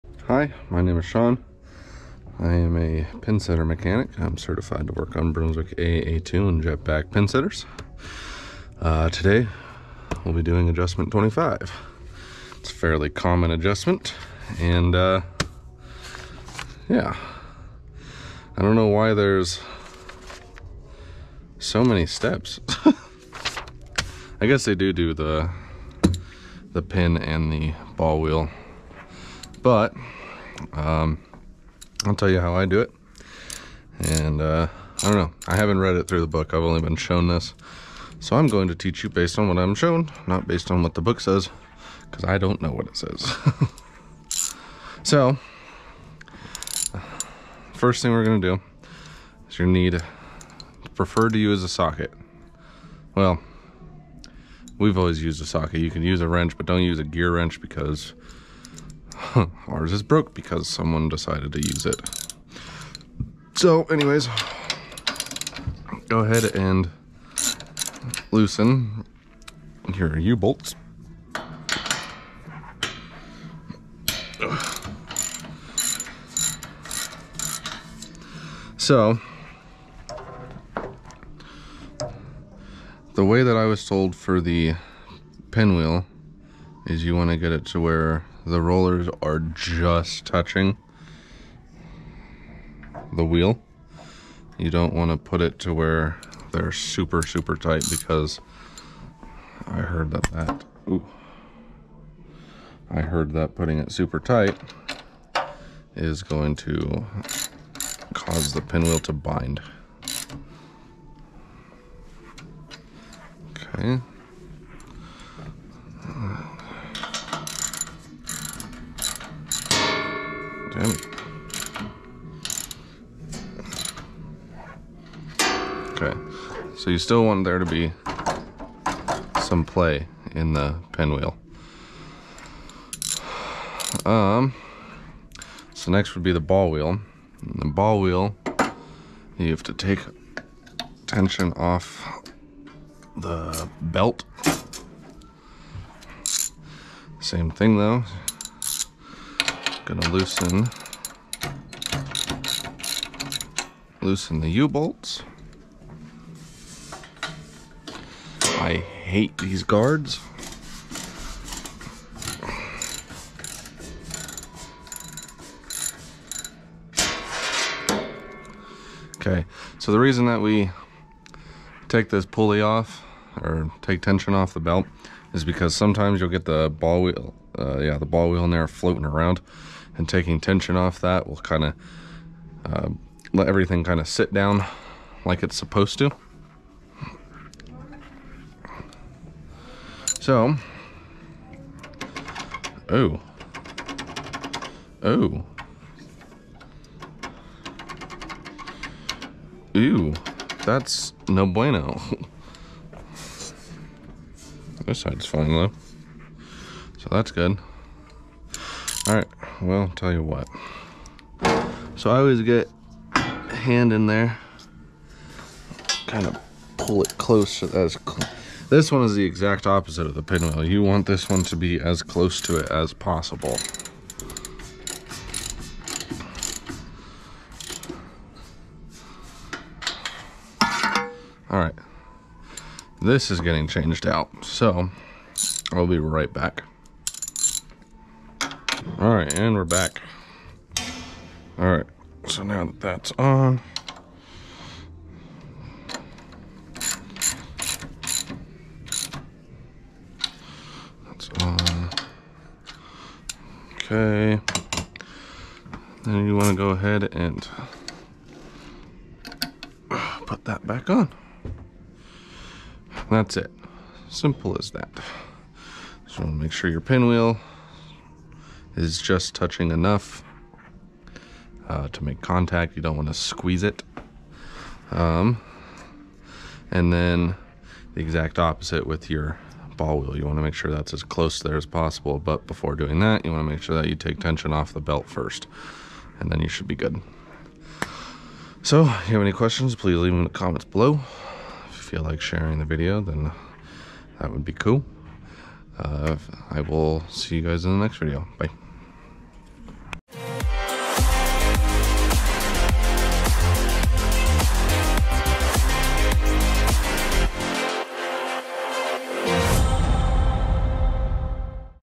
Elevator Guide Roller #25 - Brunswick Pinsetter Adjustment
Today we will be picking back up on the adjustment series